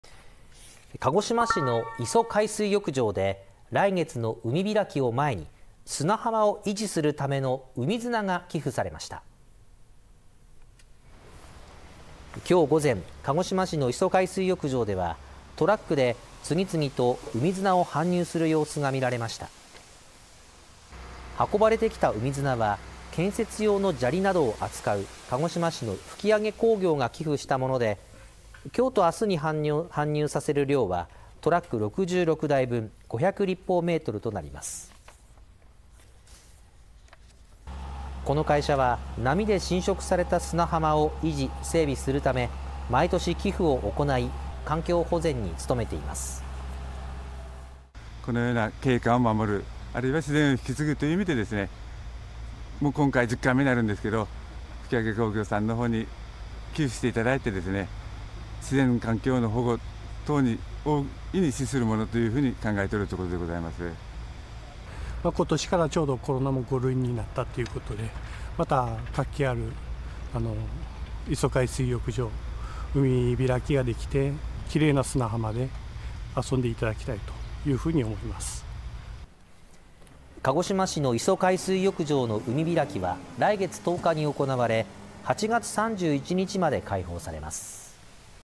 鹿児島市・磯海水浴場 海開きを前に業者が海砂を寄付 (23/06/26 21:00)
鹿児島市の磯海水浴場で７月の海開きを前に、砂浜を維持するための海砂が寄付されました。

２６日午前、鹿児島市の磯海水浴場では、トラックによって次々と海砂を搬入する様子が見られました。

運ばれてきた海砂は建設用の砂利などを扱う鹿児島市の吹上工業が寄付したもので、２６日と２７日に搬入される量はトラック６６台分、５００立方メートルになります。

この会社は波で浸食された砂浜を維持・整備するため、毎年寄付を行い環境保全に努めています。

鹿児島市河川港湾課・勝目芳生課長
「このような景観を守る、あるいは自然を引き継ぐという意味で、今回１０回目になるが吹上工業に寄付していただき自然環境の保護を維持している」

吹上工業鹿児島営業所・泉達成所長
「２０２３年から新型コロナも５類に移行したので、活気ある磯海水浴場で海開きができてきれいな砂浜で遊んでもらいたい」

鹿児島市の磯海水浴場の海開きは、７月１０日に行われ、８月３１日まで開放されます。